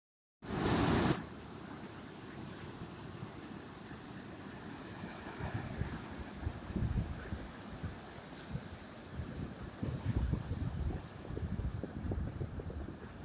Kingtides hit Seawall ☀️ 🌊 Pacific Ocean Jan 👀 incoming
Watch the seawall at the wayside

Notice the rusted pylons sticking out of the sand.  Both an eye sore and beach hazard, these remnants of iron foundation pylons was a saw mill in the old days